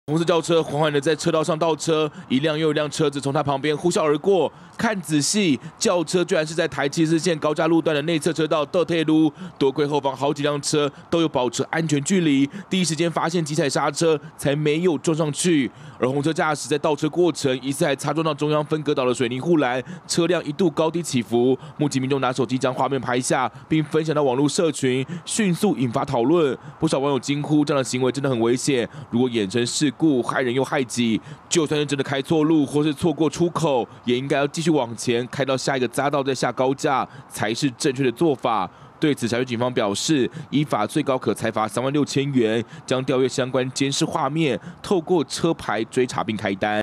台74線轎車「倒退嚕」 驚險畫面曝光引熱議【最新快訊】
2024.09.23【最新快訊】台中市有民眾直擊，台74快速道路，太平往潭子的高架路段，居然有一輛轎車，在快車道「倒退嚕」，立即拿出手將將畫面拍下，並分享到網路社群、引發熱議。